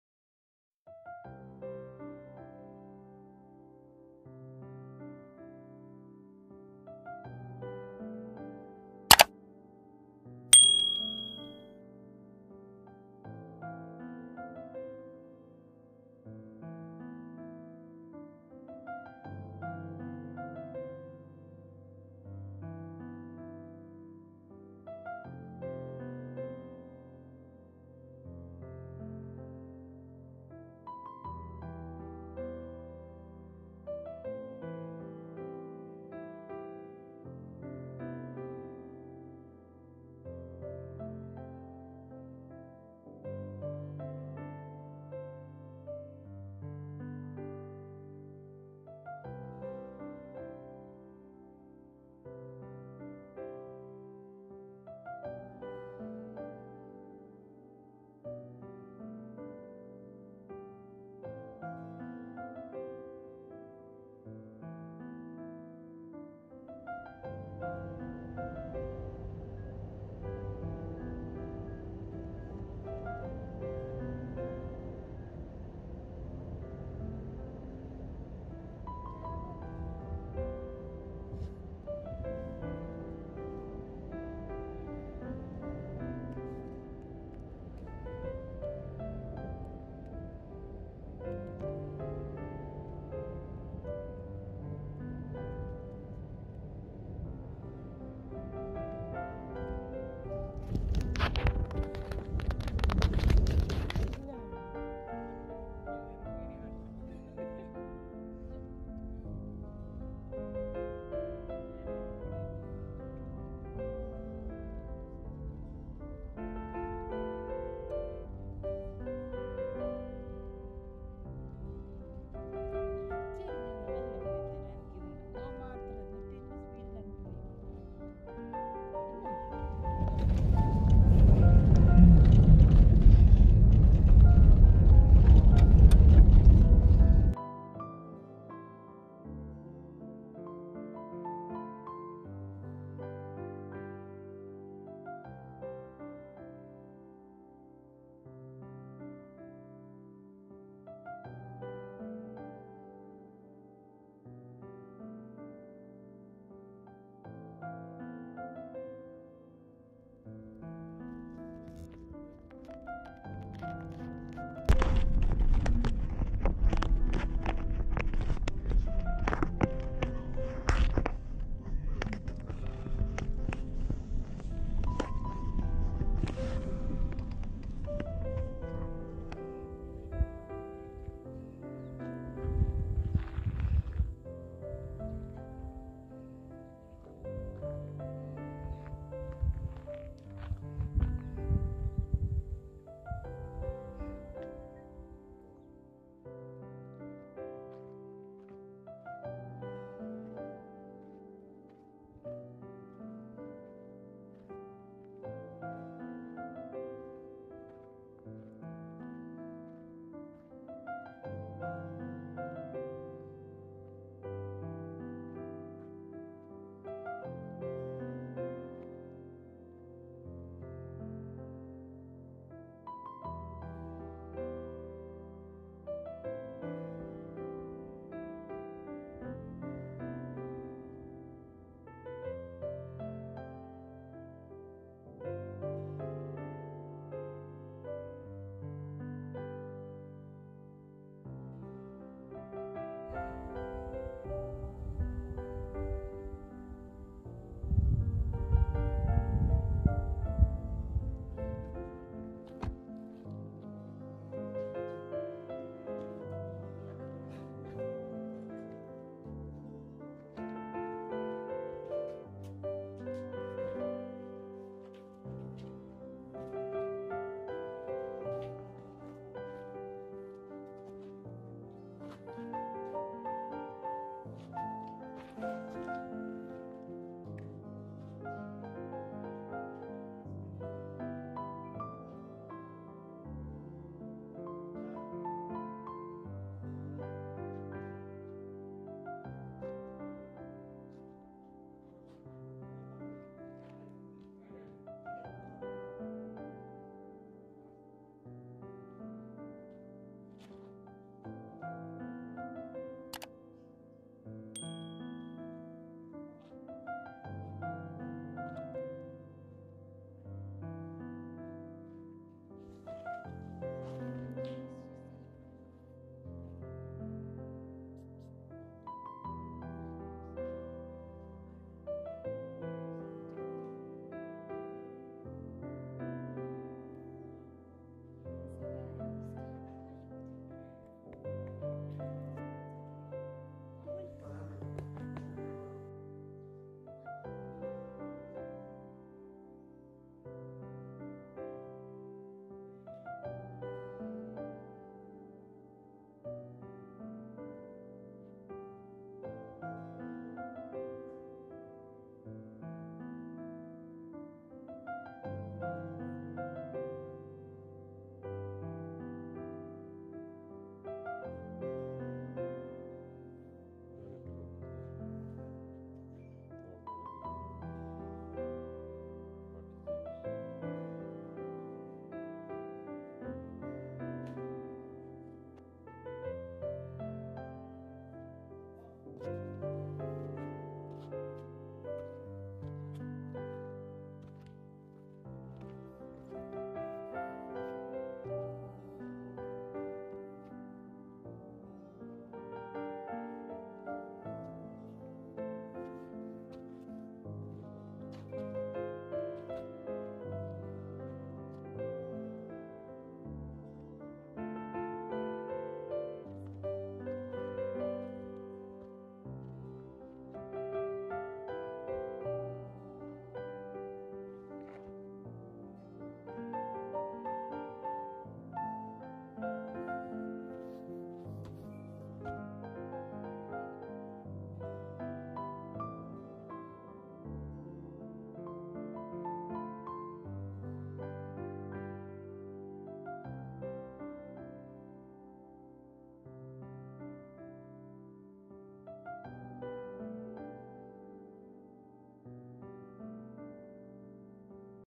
MUSFUR SINKHOLE QATAR | Places to visit in Qatar | Travel vlog
Musfur Sinkhole is believed to have formed around 300,000 years ago and is the biggest sinkhole in Qatar. Google maps can take you to the exact location.
For GPS coordinates, refer to this: N 25 10’30” E 51 12’41”

Even though Qatar is a small country, there are plenty of beaches and desert spots to explore.